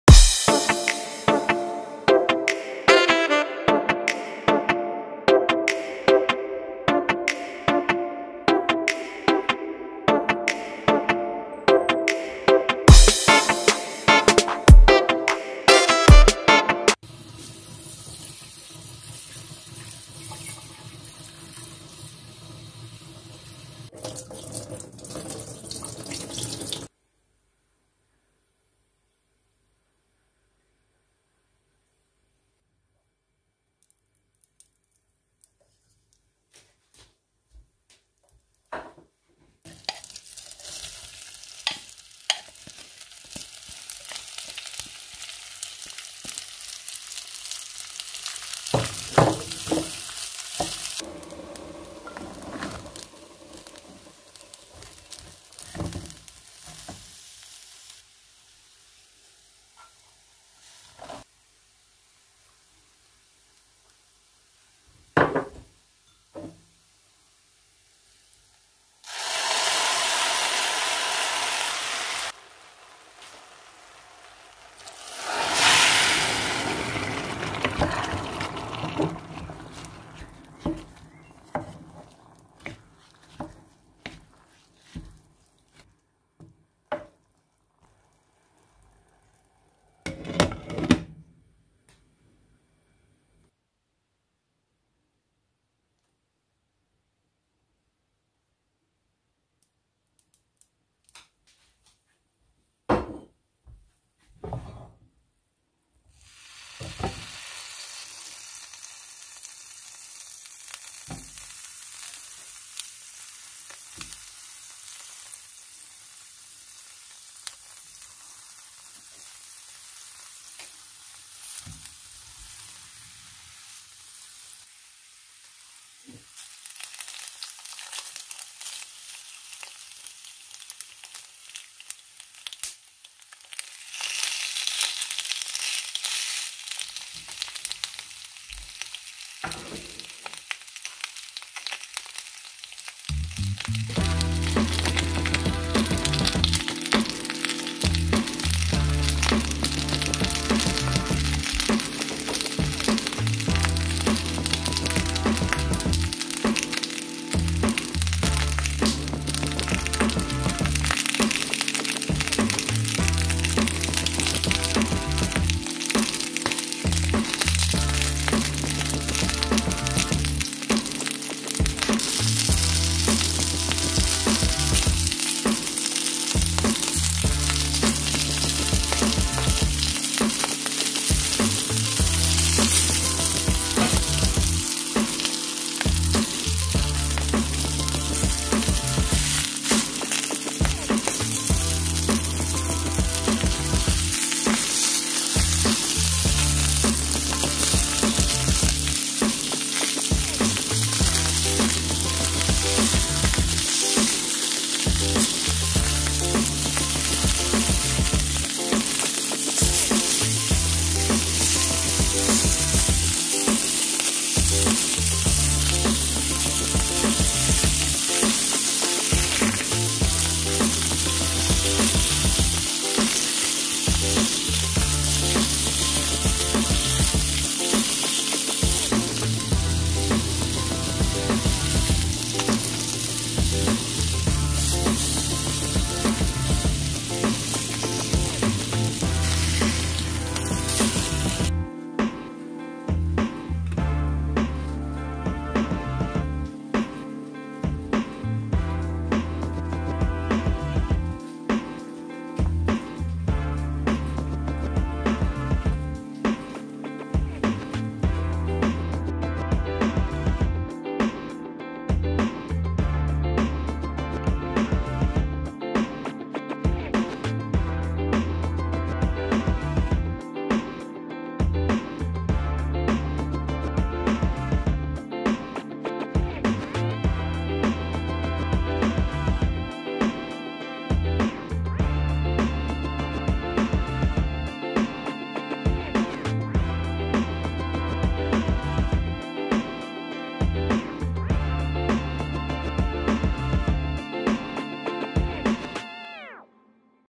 Salmon and bulgur in 7 minutes .Easy fish meal .
Salmon and bulgur is totally yummy together,  specially if you love fish .